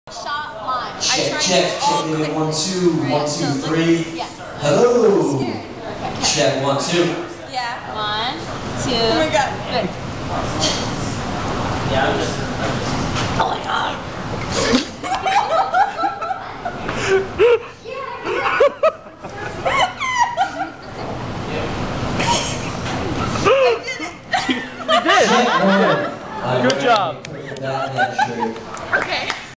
Allie's first shot of tequila!!
at McGuinn's with aly & nikki